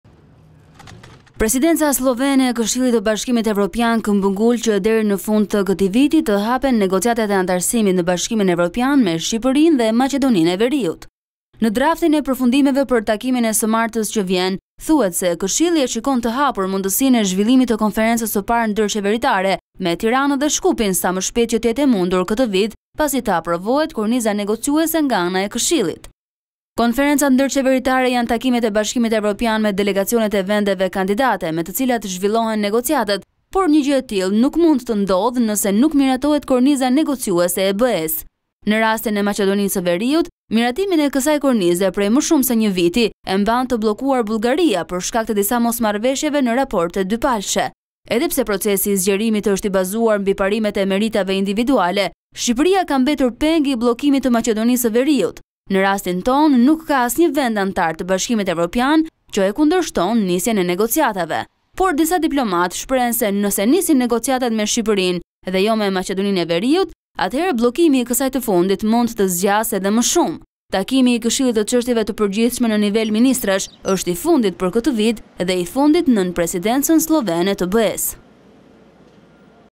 Sllovenia: Të mbahen konferencat ndërqeveritare me Shqipërinë dhe Maqedoninë e Veriut - Vizion Plus
Presidenca sllovene e Këshillit të Bashkimit Evropian këmbëngul që deri në fund të këtij viti, të hapen negociatat e anëtarësimit në BE me Shqipërinë dhe Maqedoninë e Veriut.Në draftin e përfundimeve për takimin e të martës që vjen, thuhet se “Këshilli e shikon të hapur mundësinë e zhvillimit të konferencës së parë ndërqeveritare me Tiranën dhe Shkupin, sa më shpejt që të jetë e mundur këtë vit, pasi të aprovohet korniza negociuese nga ana e Këshillit”.Konferencat ndërqeveritare janë takimet e BE-së me delegacionet e vendeve kandidate, me të cilat zhvillohen negociatat, por një gjë e tillë nuk mund të ndodhë nëse nuk miratohet korniza negociuese e BE-së.Në rastin e Maqedonisë së Veriut, miratimin e kësaj kornize prej më shumë se një viti e mban të bllokuar Bullgaria, për shkak të disa mosmarrëveshjeve në raportet dypalëshe.Edhe pse procesi i zgjerimit është i bazuar mbi parimet e meritave individuale, pra secili shtet vlerësohet individualisht, Shqipëria ka mbetur peng i bllokimit të Maqedonisë së Veriut. Në rastin e Shqipërisë nuk ka asnjë vend anëtar të BE-së që e kundërshton nisjen e negociatave.Por, disa diplomatë shprehen se nëse nisin negociatat me Shqipërinë e jo edhe me Maqedoninë e Veriut, atëherë bllokimi i kësaj të fundit mund të zgjasë edhe më shumë.Takimi i Këshillit të çështjeve të përgjithshme në nivel ministrash është i fundit për këtë vit dhe i fundit nën presidencën sllovene të BE-së.